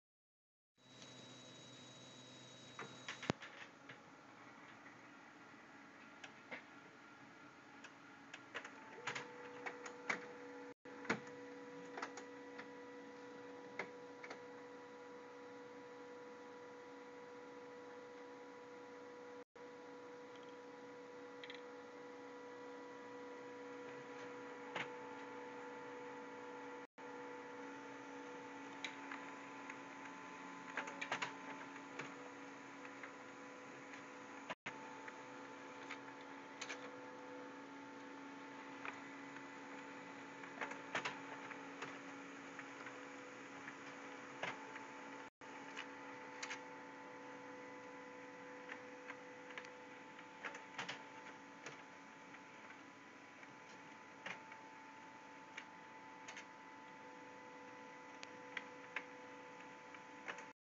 Cat is amazed by printer
My cat is just baffled by printer technology